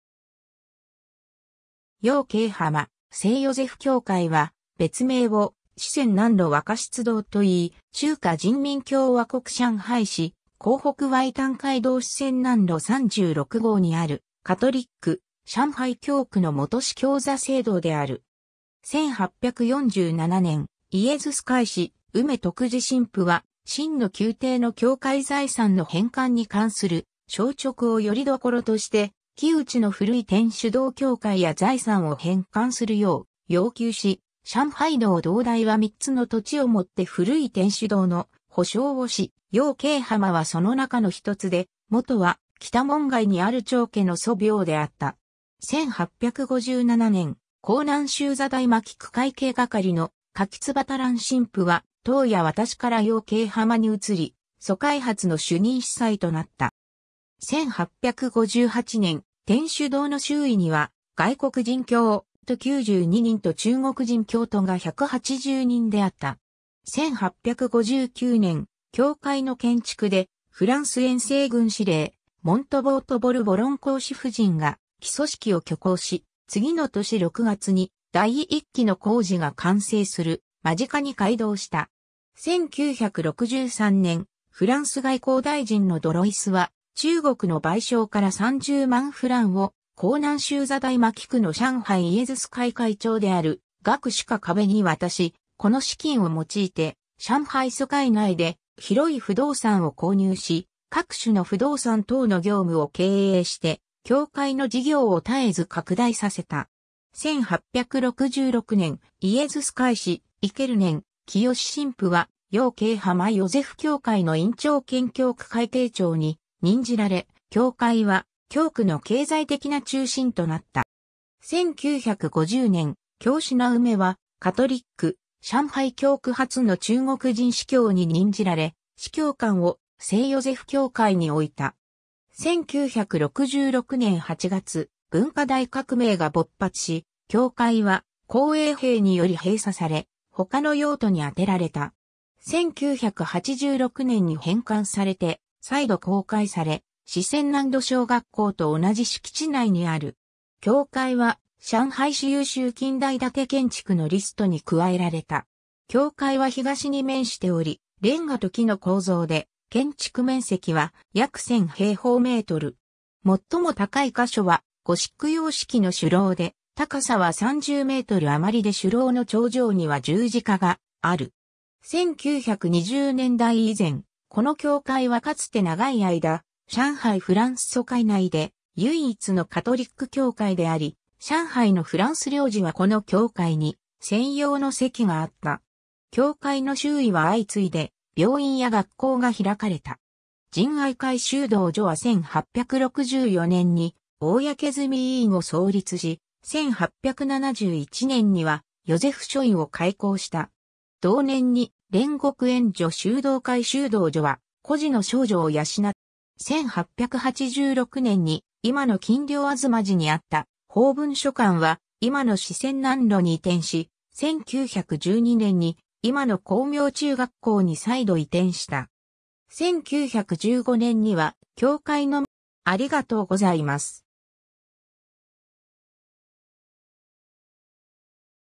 洋涇浜聖ヨゼフ教会
洋涇浜聖ヨゼフ教会（ようけいほうせいよぜふきょうかい、「浜」は音読みが「ヒン」で訓読みが「はま」で濱の新字体として使われる漢字ではなく、音読みが「ホウ」で川を意味する漢字である。中国語：洋涇浜聖若瑟堂）は、別名を四川南路若瑟堂といい、中華人民共和国上海市黄浦区外灘街道四川南路36号にあるカトリック上海教区の元司教座聖堂である。
 1847年（道光26年）、イエズス会士梅徳爾（Lemaitre Mathurin）神父は、清の宮廷の教会財産の返還に関する詔勅を拠り所として、城内の古い天主堂教会や財産を返還するよう要求し、上海道道台は3つの土地を以て古い天主堂の補償をし、洋涇浜はその中の一つで、元は北門外にある張家の祖廟であった。
 1857年（咸豊7年）、江南宗座代牧区会計係の杜若蘭（Desjacques Marin）神父は董家渡から洋涇浜に移り、租界初の主任司祭となった。
 1858年（咸豊8年）、天主堂の周囲には外国人教徒92人と中国人教徒が180人であった。
 1859年（咸豊10年）、教会の建築で、フランス遠征軍司令モントボー（Montaubaw）とボルボロン（Bourbou1on）公使夫人が基礎式を挙行し、次の年6月に第一期の工事が完成する間近に開堂した。
1963年（同治2年）、フランス外交大臣のドロイスは中国の賠償から30万フランを江南宗座代牧区の上海イエズス会会長である鄂爾壁に渡し、この資金を用いて上海租界内で広い不動産を購入し、各種の不動産等の業務を経営して、教会の事業を絶えず拡大させた。
 1866年（同治5年）、イエズス会士蘇念澄神父（Basuiau Hippolyte）は、洋涇浜ヨゼフ教会の院長兼教区会計長に任じられ、教会は教区の経済的な中心となった。
 1950年、龔品梅はカトリック上海教区初の中国人司教に任じられ、司教館を聖ヨゼフ教会に置いた。
 1966年8月、文化大革命が勃発し、教会は紅衛兵により閉鎖され、他の用途に充てられた。
 1986年に返還されて再度公開され、四川南路小学校と同じ敷地内にある。
教会は上海市優秀近代建建築のリストに加えられた。
 教会は東に面しており、レンガと木の構造で、建築面積は約1,000平方メートル。
最も高い箇所はゴシック様式の鐘楼で、高さは30メートル余りで鐘楼の頂上には十字架がある。
 1920年代以前、この教会はかつて長い間、上海フランス租界内で唯一のカトリック教会であり、上海のフランス領事はこの教会に専用の席があった。
 教会の周囲は相次いで病院や学校が開かれた。
仁愛会修道女は1864年（同治3年）に公済医院を創立し（後に北蘇州路に移転）、1871年（同治10年）にはヨゼフ書院（外僑女子中学校）を開校した。
同年に煉獄援助修道会修道女は、孤児の少女を養って学校を運営し、1875年（光緒元年）に上智女校（即ち暁明女子中学校、市六女子中学校、現在の旅遊職業学校）とヨゼフ女子孤児院（多くはヨーロッパ人とアジア人の混血児で、1937年に徐家匯聖イグナチオ大聖堂に移転した）に分け、1874年（同治13年）には聖フランシスコ学院を創立し、1876年（光緒2年）には煉獄援助修道会女子修道院（別名聖母院、1959年に総会に合併された）の建造を開始した。
1886年（光緒12年）に今の金陵東路にあった法文書館（即ち中法学堂）は、今の四川南路に移転し、1912年（民国2年）に今の光明中学校に再度移転した。
1915年（民国5年）には教会の前の北側にルイス小学校（今の四川南路小学校で、教会の経営していた中小学校は、中華人民共和国成立後に教育部門により接収された）を開いた。